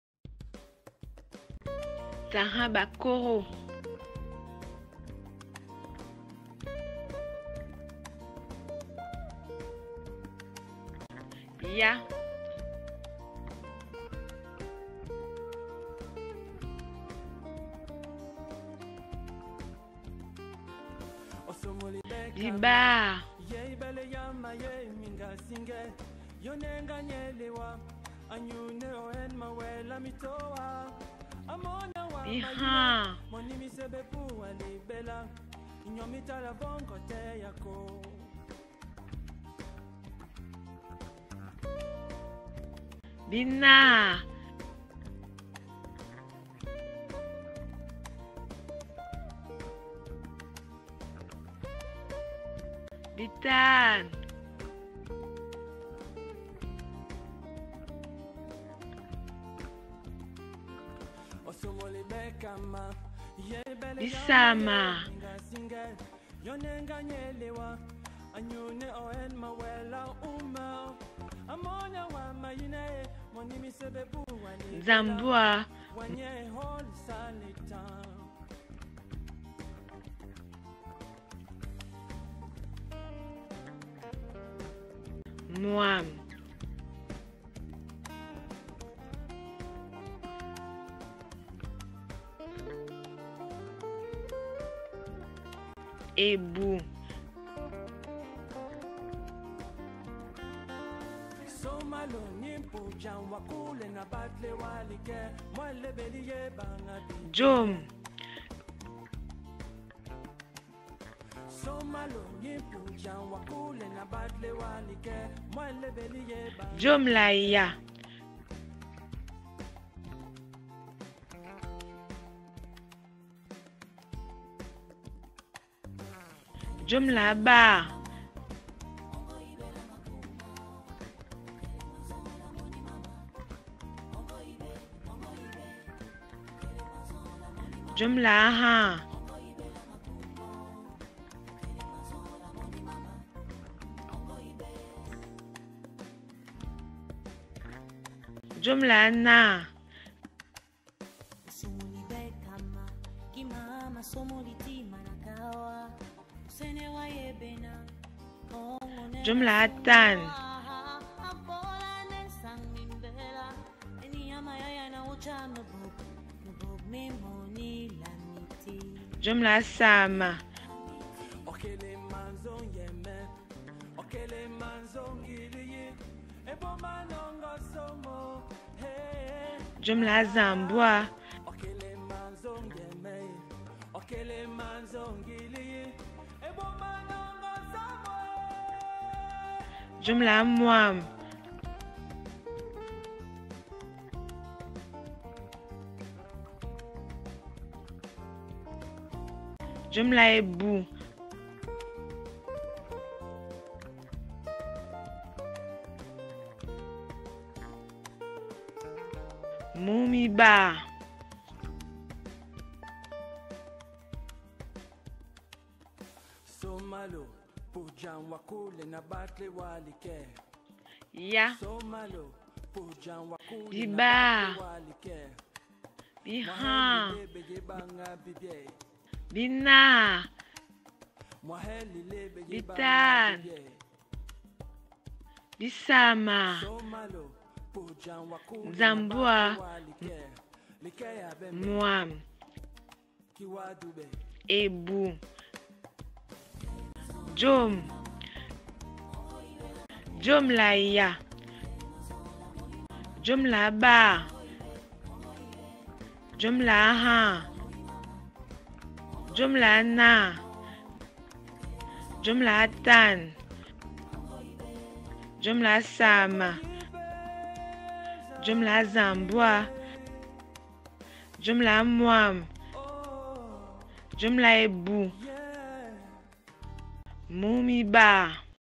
Les Chiffres de 1 à 20 en langue BAKOKO
video d'apprentissage et de prononciation des chiffres en Langue BAKOKO (Táŋga ɓakógō 1 - 20 ).

Le Bakoko est une des langues locales du Littoral du.Cameroun